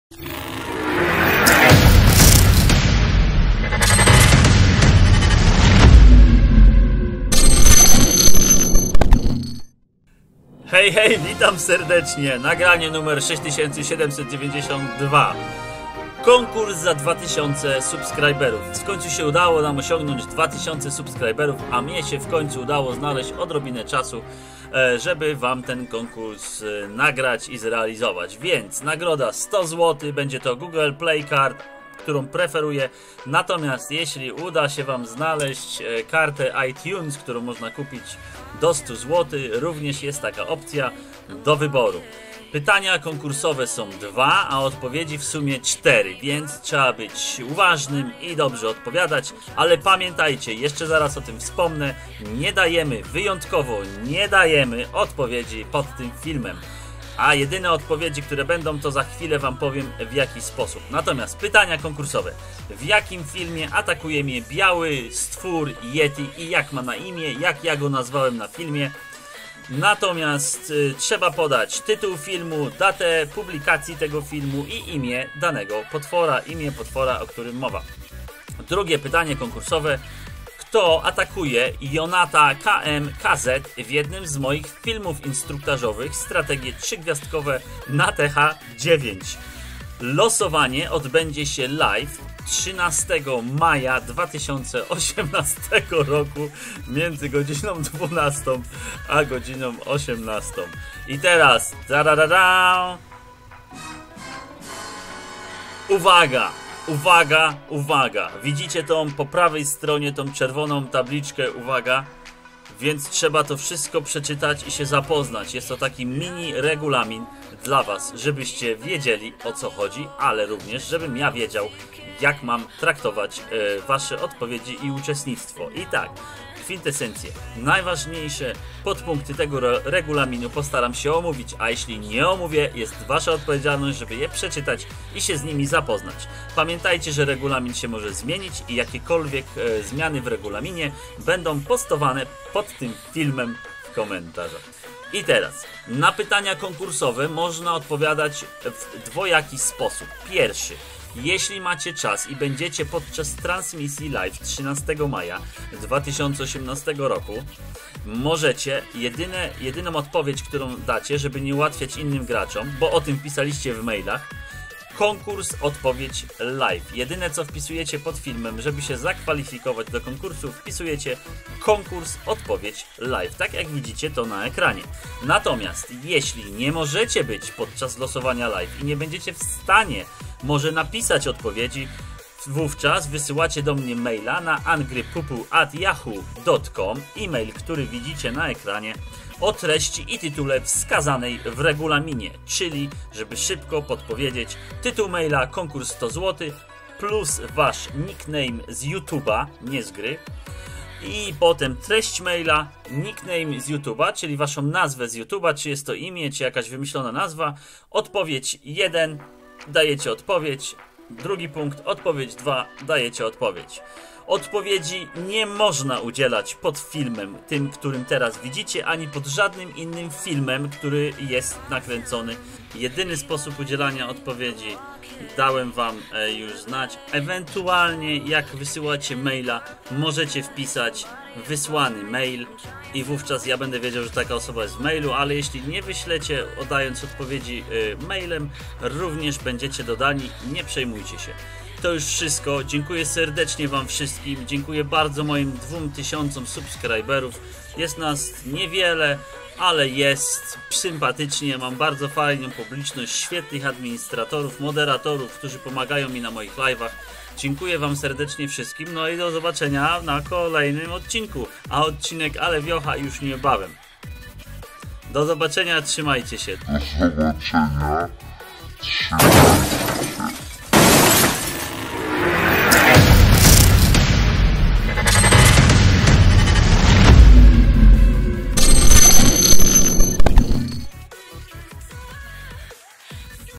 🔥🔥🔥 KONKURS 100 zl za 2k SUBSCRIBERS - CONTEST 2K 🔥🔥🔥
Witam
Zapraszam na losowanie nagrody 100 zl karty google play lub karty ITUNES ktora da sie kupic uzywajac 100 zl -

Pytania Konkursowe :
1.W jakim filmie atakuje mnie bialy Yeti i jak ma na imie ?
(tytul filmu , data publikacji , Imie Yeti )
2.Kto atakuje JONATA KMKZ w jednym z moich
 filmow strategie 3 gwiazdkowe na TH9.
Losowanie odbedzie sie Live 13/05/2018
(miedzy godzina 12:00 a 18:00)

UWAGA WAZNE!!! - nie udizelamy odpowiedzi na pytania pod Filemem na YT - czytajcie ponizej-

-Na ptania konkuroswe mozna odpowiedziec
podczas live transmisji -13 Maja
Aby wziasc udzial w konkursie  wystarczy wpisac "KONKURS - ODPOWIEDZ LIVE " w komentarzu pod tym filmem zas odpowiedzi bedzie trzeba udzielic po wylosowaniu.

Tytul maila: KONKURS 100 zl + wasz nick name z YOUTUBA
Tresc maila:
Nick z Youtuba
1.Odpowiedz na pytanie 1.:""
2.Odpowiedz na pytanie 2.:22

-Odpowiedzi nie mozna udzielac pod filmem - Kazda osoba wpisujaca poprawne odpowiedzi  w komentarzach pod filmem - automatycznie zostanie wykluczona z konkursu.
-zwyciazkie konto bedzie podlegac weryfikacji we wskazany przeze mnie sposob.
-calkowita i wieczysta dyskfalifikacja za multi konta.
-Zastrzegam sobie prawo do zmiany regulaminu - wszelkie uaktualnienia beda umieszczone w komentarzu pod filmem.

Powodzenia

Pamietajcie o Lapkach - w gore  no i o dzwoneczkach 🍺🍺🍺